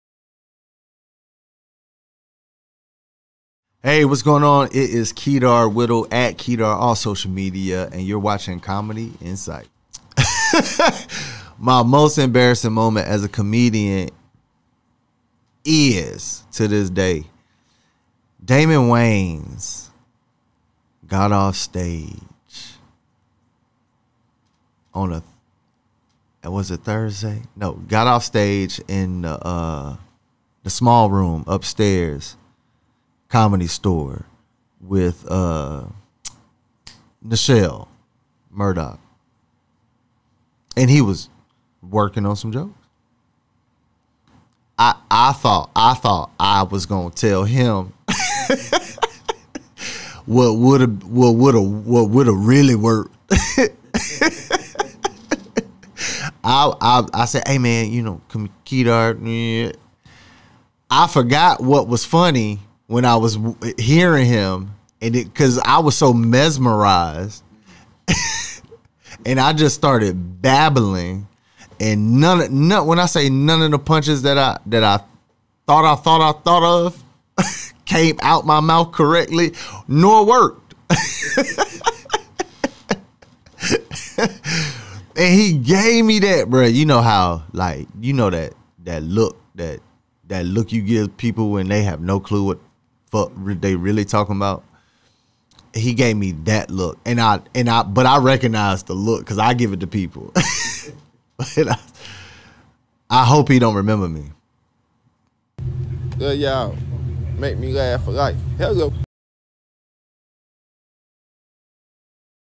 I was mesmerized by Damon Wayans and just babbled!
@keedarwhittle had a not so memorable moment with Damon Wayans for his most embarrassing moment on Comedy Insights.

WHO SHOULD WE INTERVIEW NEXT?